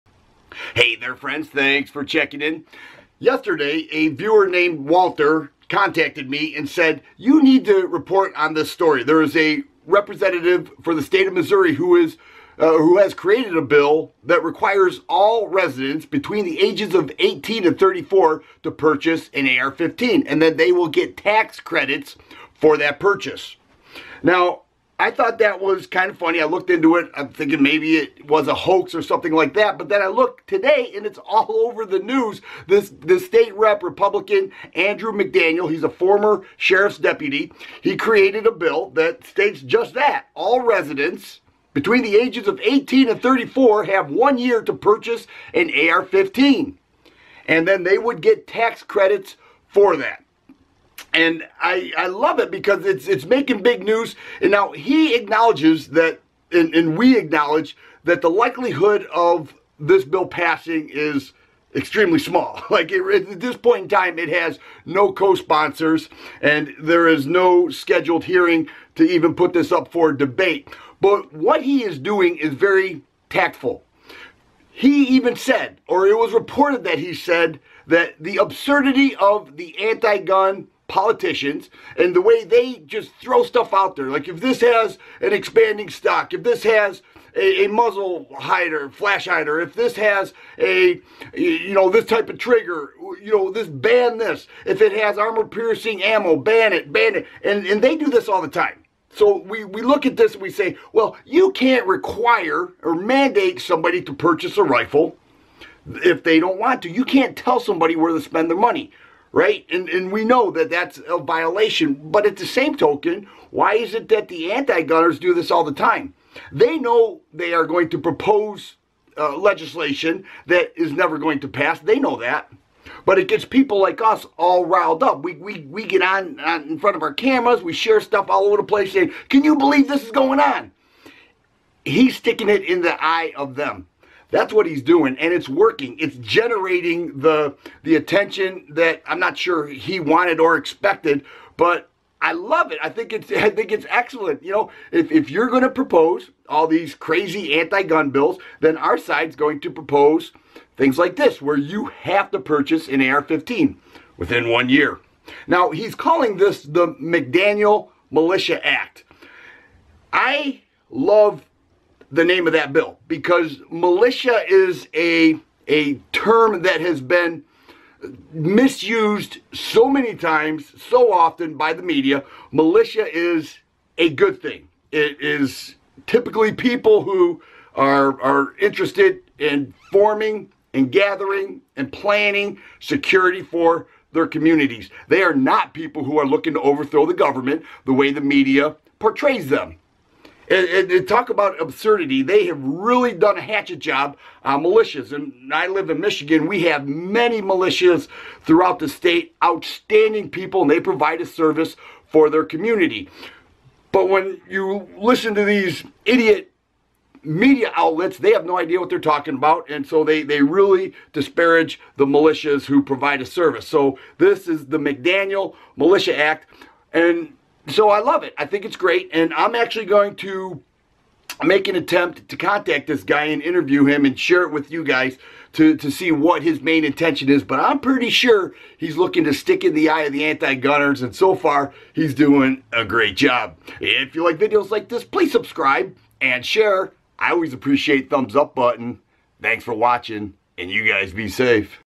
Missouri Rep says "You MUST Buy an AR15" - TheFireArmGuy
Missouri Rep Andrew McDaniel wrote a bill that mandates 18 - 34 year old residents must buy an AR15. This bill is called the McDaniel Militia Act. If passed, the state would offer tax credits for the purchase of their AR15 rifles.